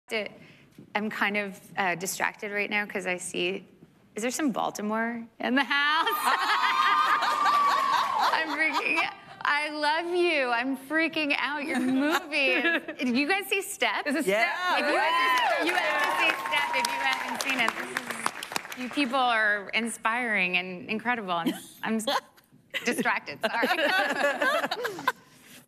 Natalie Portman Loves the Lethal Ladies
Natalie Portman can't contain her excitement that the Lethal Ladies of BLSYW are in the room.